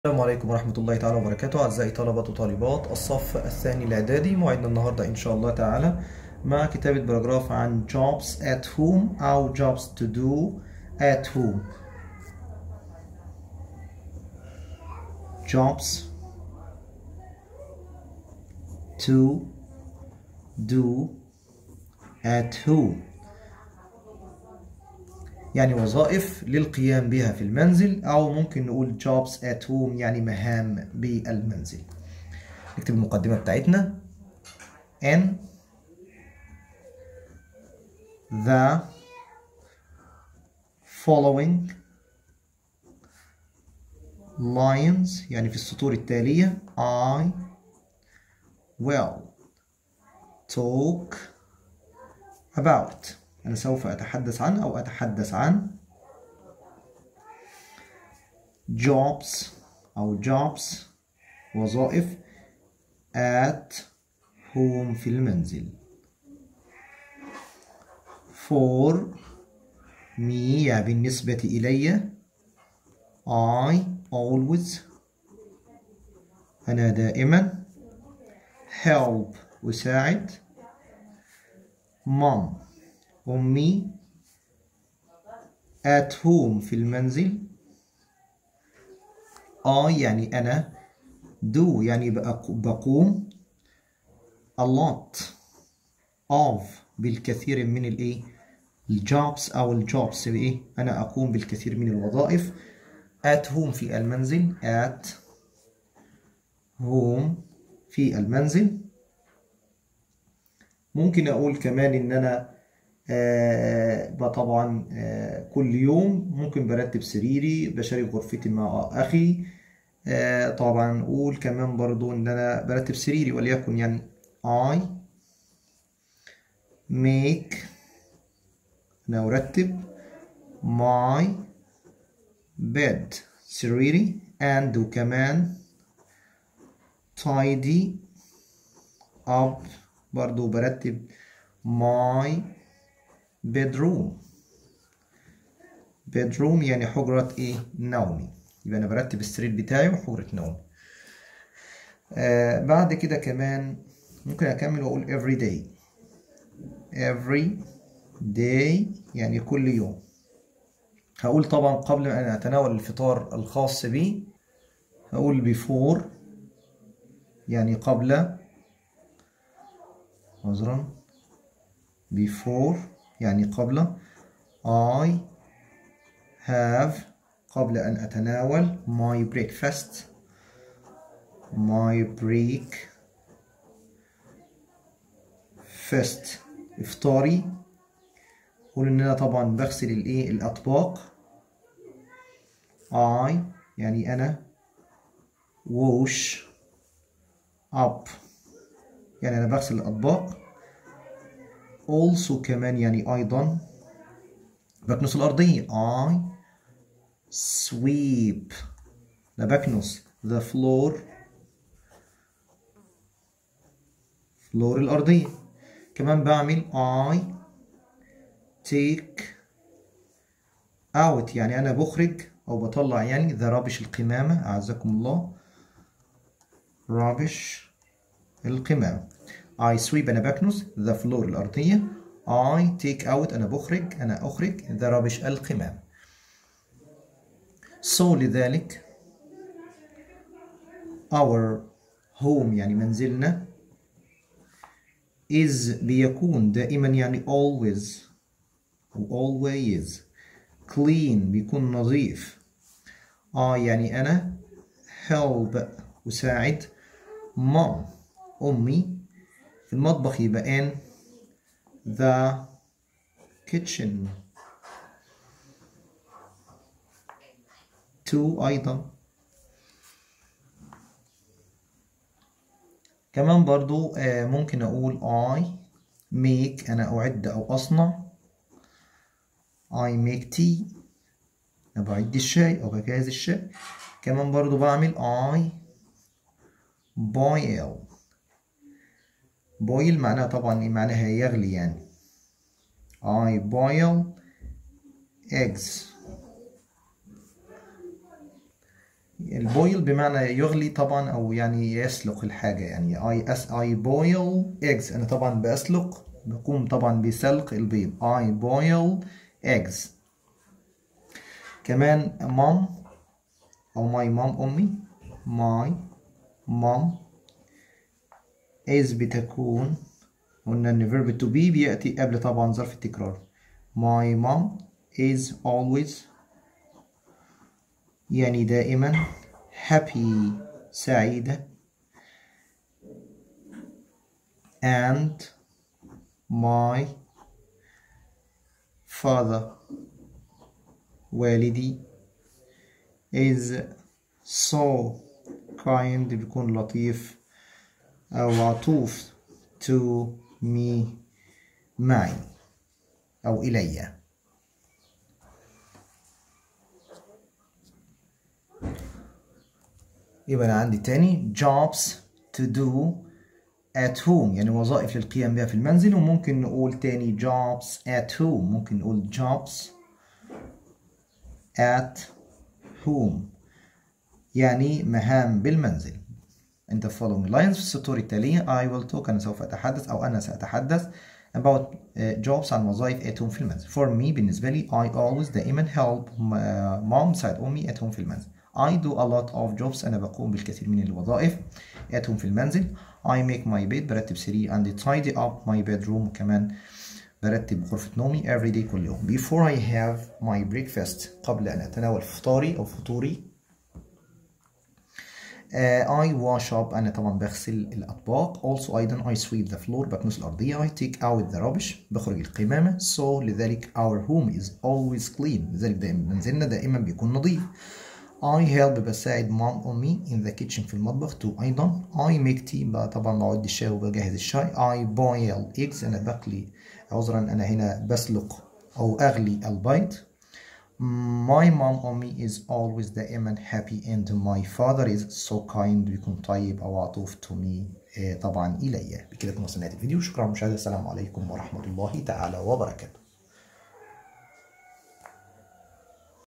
ازاي تكتب براجراف تانيه اعدادي انجليزي2024 عن jobs to do at home مراجعة paragraph |تانية_ اعدادي
اهم براجرافات الصف الثاني الاعدادي english. براجراف عن روتيني اليومي my daily routine للصف الثاني الاعدادي ترم اول 2024
رياضتى المفضله My favourite sport براجراف عن.مواهب نور الإسلام